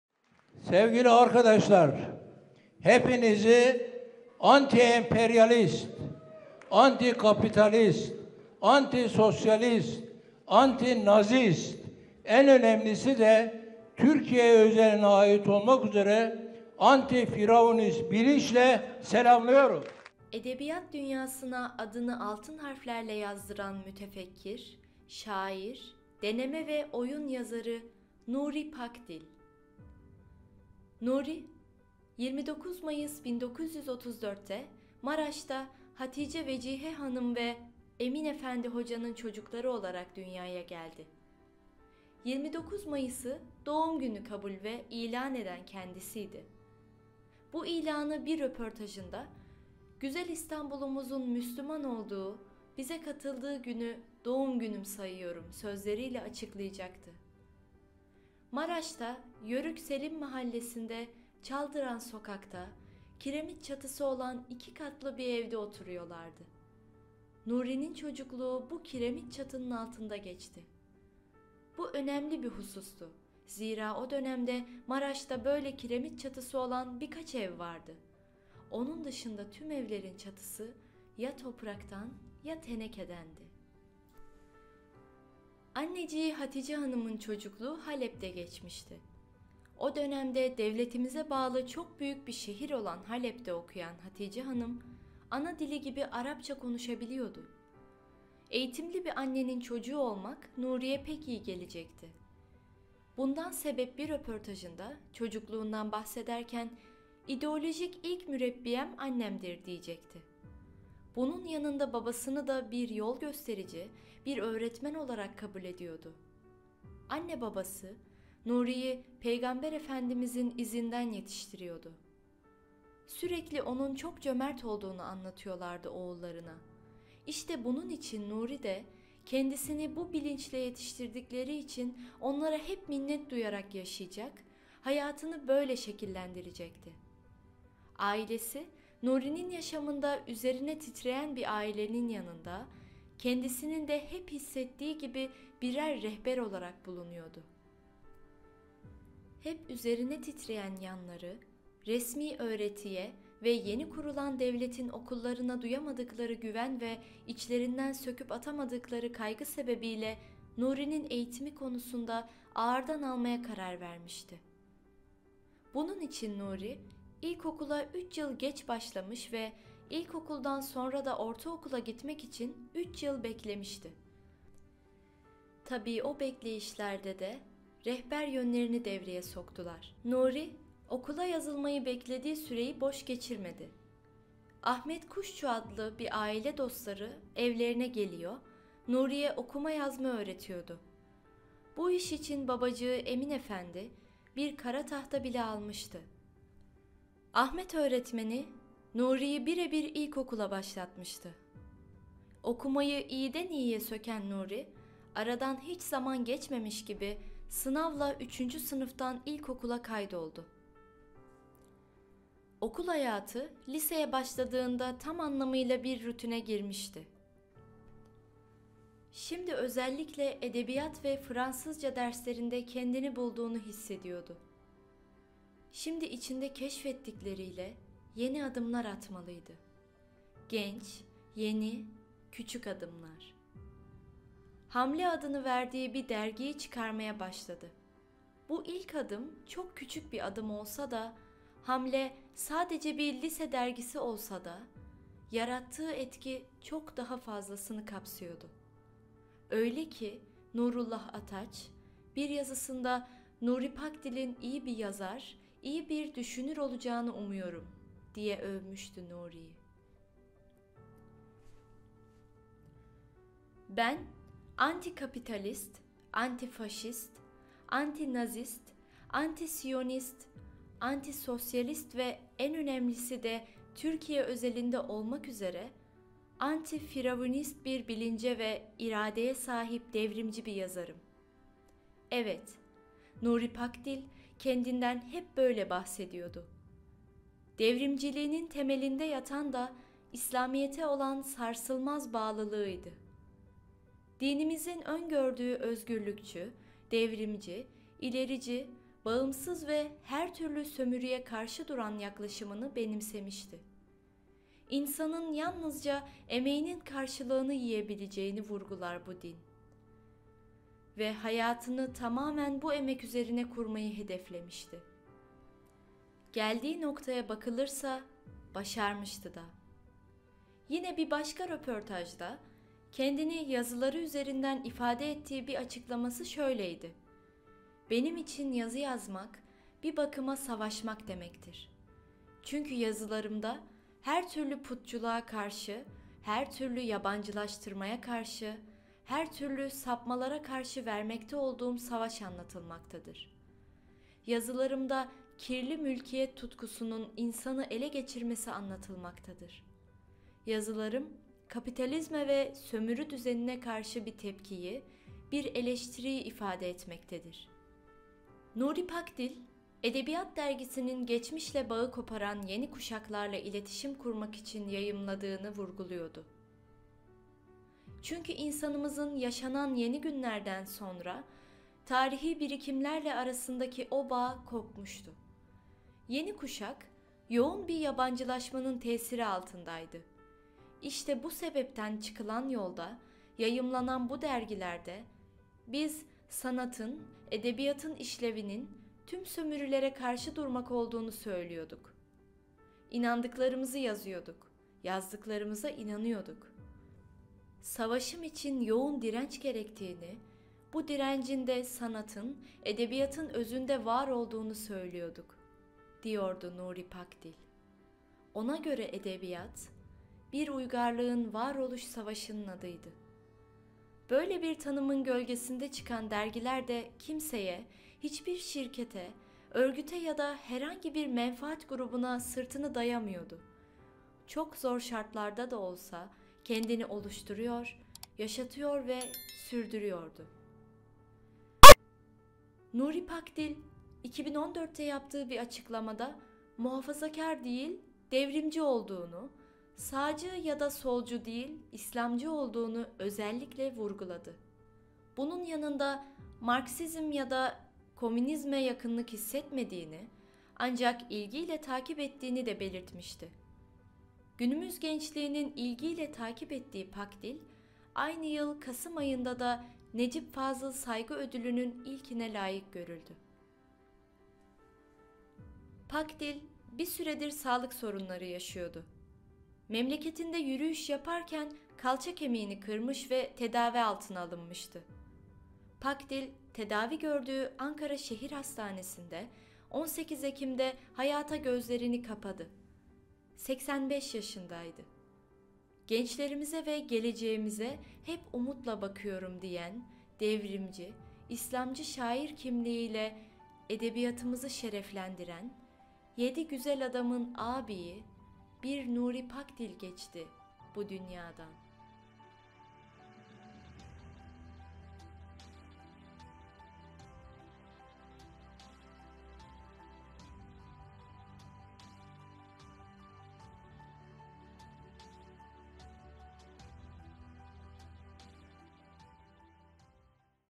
Kudüs Şairi Nuri Pakdil Aslında Kimdir? | Yedi Güzel Adamdan Biri Olan Nuri Pakdil Kim?
Edebiyat dünyasına adını altın harflerle yazdıran mütefekkir, şair, deneme ve oyun yazarı Nuri Pakdil.
Nuri, 29 Mayıs 1934’te Maraş’ta, Hatice Vecihe Hanım ve Emin Efendi Hoca’nın çocukları olarak dünyaya geldi. 29 Mayıs’ı doğum günü kabul ve ilan eden kendisiydi. Bu ilanı bir röportajında “Güzel İstanbul’umuzun Müslüman olduğu, bize katıldığı günü, doğum günüm sayıyorum.” sözleriyle açıklayacaktı. Maraş’ta, Yörük Selim Mahallesi’nde, Çaldıran Sokak’ta, kiremit çatısı olan iki katlı bir evde oturuyorlardı. Nuri’nin çocukluğu bu kiremit çatının altında geçti. Bu önemli bir husustu; zira o dönemde Maraş’ta böyle kiremit çatısı olan birkaç ev vardı. Onun dışında tüm evlerin çatısı ya topraktan ya tenekedendi.
Anneciği Hatice Hanım’ın çocukluğu Halep’te geçmişti. O dönemde devletimize bağlı çok büyük bir şehir olan Halep’te okuyan Hatice Hanım, ana dili gibi Arapça konuşabiliyordu. Eğitimli bir annenin çocuğu olmak Nuri’ye pek iyi gelecekti. Bundan sebep bir röportajında çocukluğundan bahsederken, “İdeolojik ilk mürebbiyem annemdir.” diyecekti. Bunun yanında babasını da bir yol gösterici, bir öğretmen olarak kabul ediyordu. Anne babası, Nuri’yi, Peygamber Efendimiz (s.a.v) izinden yetiştiriyordu. Sürekli onun çok cömert olduğunu anlatıyorlardı oğullarına. İşte bunun için Nuri de, kendisini bu bilinçle yetiştirdikleri için onlara hep minnet duyarak yaşayacak, hayatını böyle şekillendirecekti. Ailesi, Nuri’nin yaşamında üzerine titreyen bir ailenin yanında, kendisinin de hep hissettiği gibi birer rehber olarak bulunuyordu. Hep üzerine titreyen yanları, resmi öğretiye ve yeni kurulan devletin okullarına duyamadıkları güven ve içlerinden söküp atamadıkları kaygı sebebi ile Nuri’nin eğitimi konusunda ağırdan almaya karar vermişti. Bunun için Nuri, ilkokula 3 yıl geç başlamış ve ilkokuldan sonra da ortaokula gitmek için 3 yıl beklemişti. Tabii o bekleyişlerde de rehber yönlerini devreye soktular. Nuri, okula yazılmayı beklediği süreyi boş geçirmedi. Ahmet Kuşçu adlı bir aile dostları evlerine geliyor, Nuri’ye okuma yazma öğretiyordu. Bu iş için babacığı Emin Efendi, bir kara tahta bile almıştı.